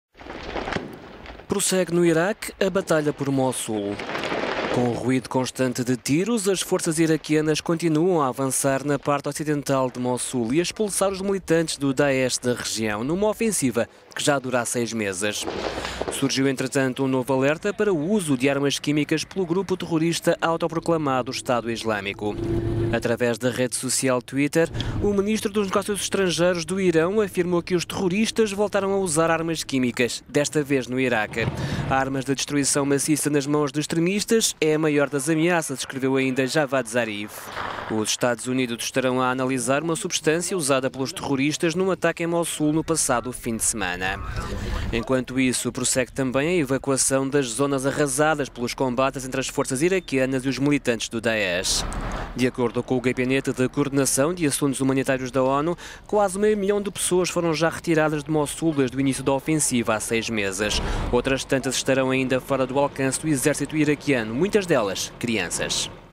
Batalha por Mossul: Exército iraquiano arrasa jiadistas e retirada de civis prossegue
Prossegue no Iraque a batalha por Mossul. Com o ruído constante de tiros, as forças iraquianas continuam a avançar na parte ocidental da cidade e a expulsar os militantes do "daesh" da região, numa ofensiva que dura já há seis meses.

Os relatos do exército apontam para a reconquista de diversos bairros, com apoio da força aérea iraquiana e da coligação internacional liderada pelos Estados Unidos. Há também o registo da morte de vários responsáveis militares dos jiadistas e da destruição de dive…